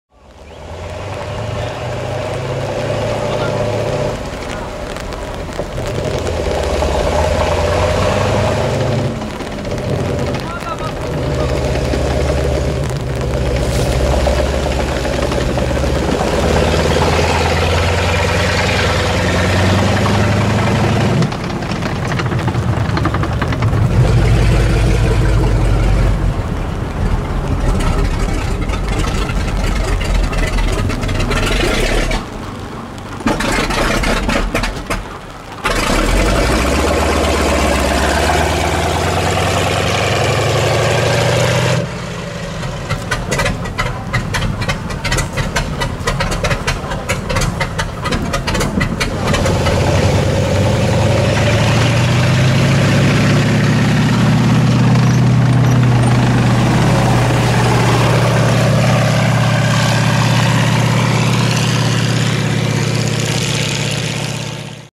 M4 SHERMAN "squirrel" radial engine irregular sound !!!
Continental R-975 C4 radial engine.

This year (2013) this sherman engine passed a large inspection and works more stably at low speed :)